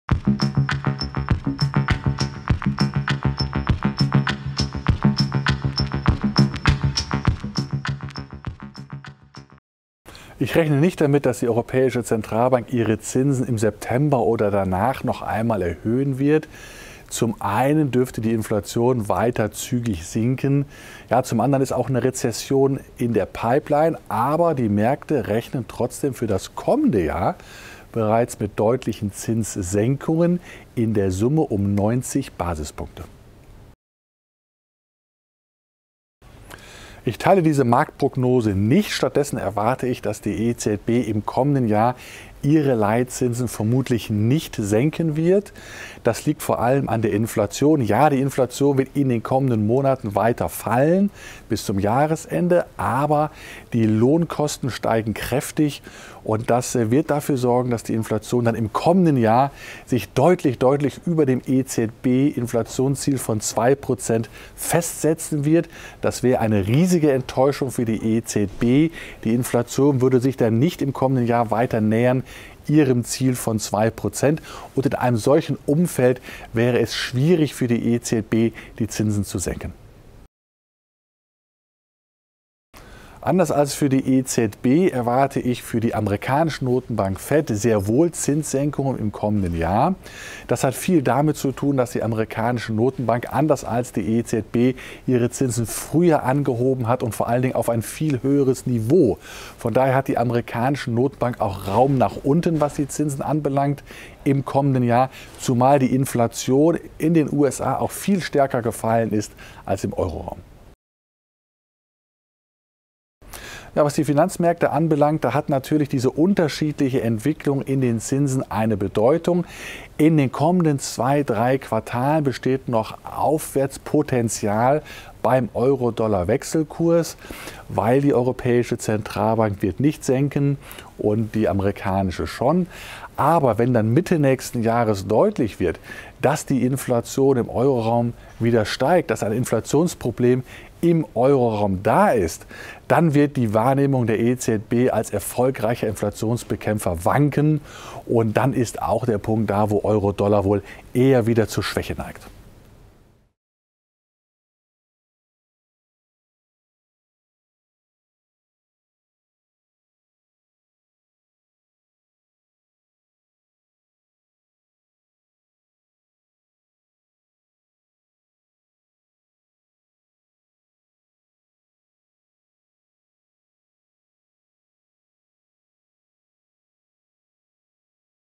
Nicht auf EZB-Zinssenkungen wetten
11. August 2023: Viele Anleger spekulieren schon seit langem, dass die EZB nächstes Jahr die Zinsen deutlich senkt.